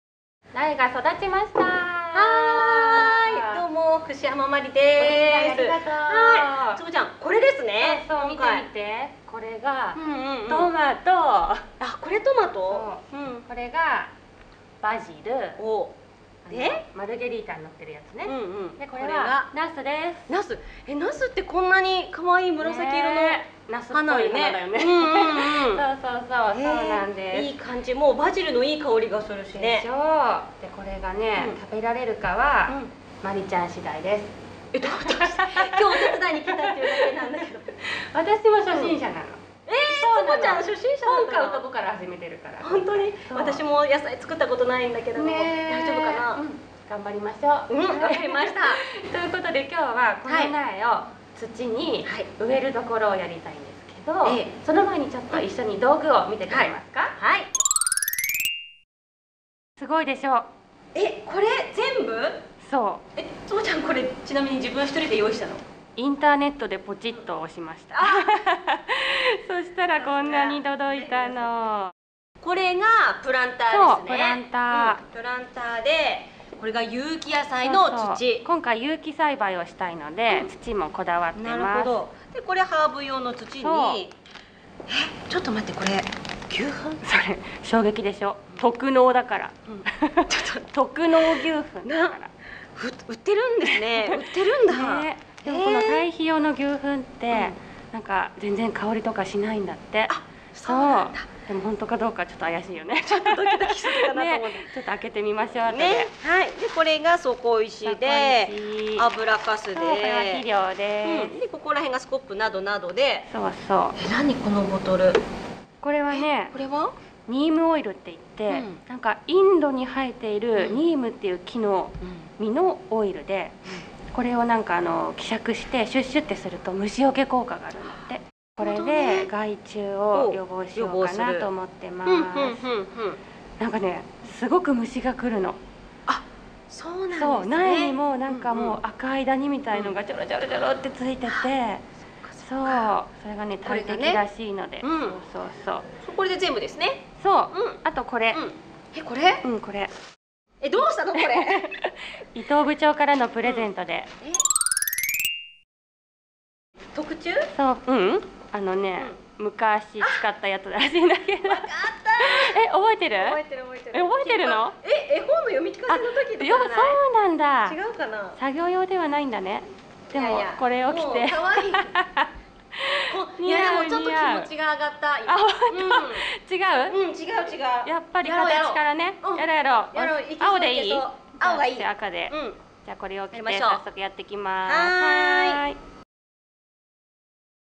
会社で野菜を育てたい！第2回「まずは形から・・」【HOMEアナウンサー】
▼HOMEぽるぽるTV「アナウンサーチャンネル」

■「会社で野菜を育てたい！」とは？
広島ホームテレビ#アナウンサー の#坪山奏子 が#家庭菜園 ならぬ社内菜園を行います！
一息つけるようなほっこり内容になってます。

■出演
串山 真理（HOMEアナウンサー）
坪山 奏子（HOMEアナウンサー）